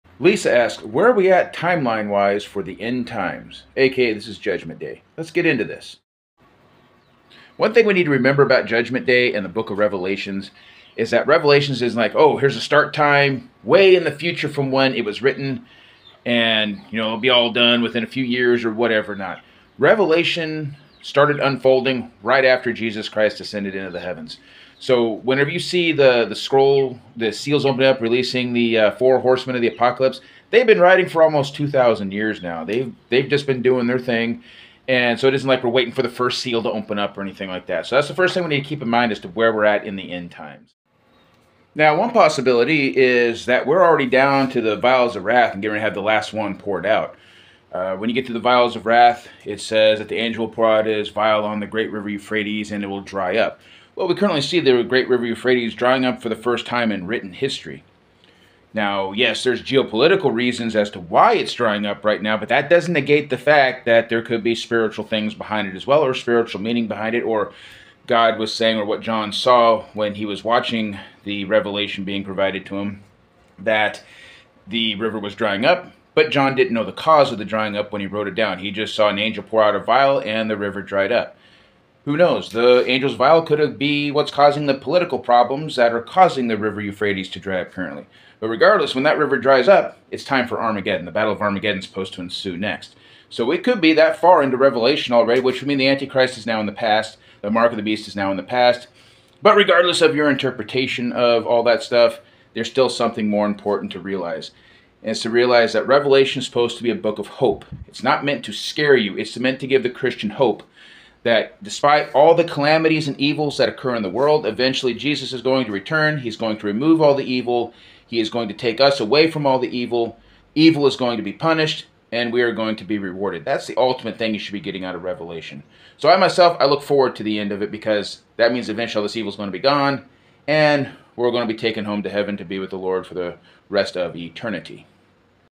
Where are we in Revelation
Revelation is a complex text to interpret correctly.  We could technically be anywhere in thar text timeline wise.  The best practice is to live like we are out of time and have plenty of time at the same time.  That is, live like you will die tomorrow while living like you will live tomorrow.  Or, hope for the best while preparing for the worst.

Revelation is not meant to be a scary book.  When I was young reading it for the first time, it was scary.  As I have grown in Christ, I have come to realize that the purpose of the book is to give hope.  The hope in the return of Christ Jesus.  The hope in the ultimate destruction and punishment of evil.  The hope of being united with the Father in Heaven.

As I delved deeper in study, I learned that this text would have been comforting to the very early Church.  Remember, very early Christian were mocked, tortured, imprisoned, and killed for their faith.  Revelation would have told them that all who harmed you for your faith in Jesus will incur the full wrath, judgement, and punishment of the Almighty GOD while those in Jesus the Christ will experience the totality of GOD's love, compassion, and will eternal be childern of the One True GOD.